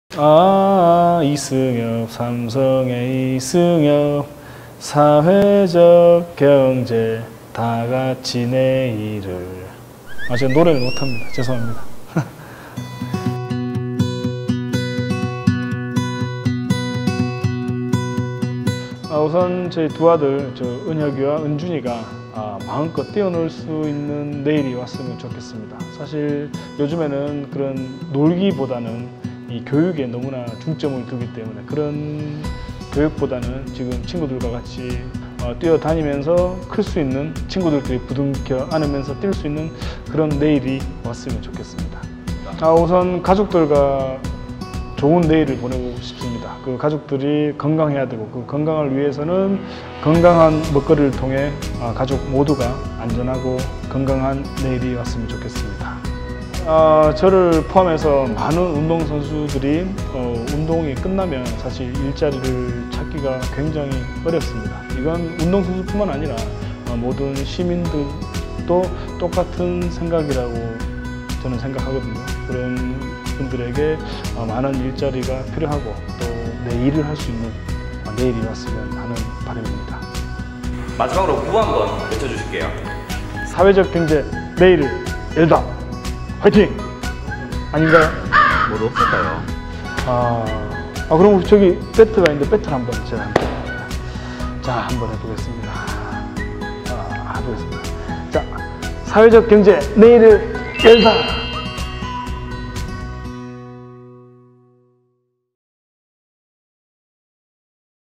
라이온킹 이승엽이 말하는 사회적경제
전 삼성라이온즈 선수이자 '국민타자'인 이승엽 선수가
사회적경제와 2018 사회적경제박람회를 응원합니다.
(이승엽 선수는 대구시 홍보대사랍니다.)

이승엽 선수가 말하는 사회적경제 이야기 들어보실래요?